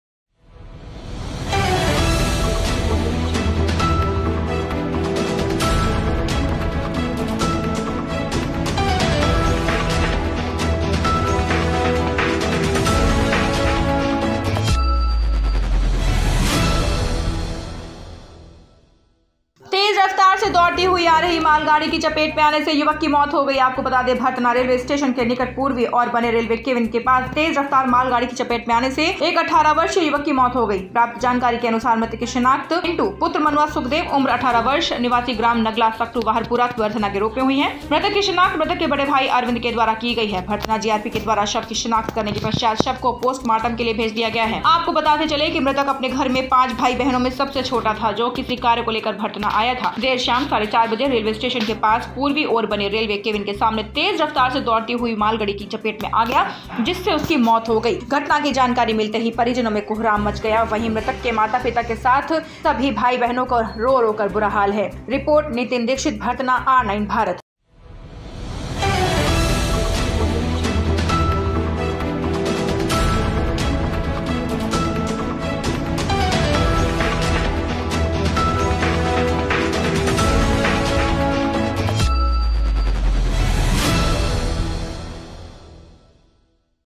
मालगाड़ी की चपेट में आकर युवक की हुई मौत, परिजनो का रो रो कर बुरा हाल
नितिन दीक्षित भरथना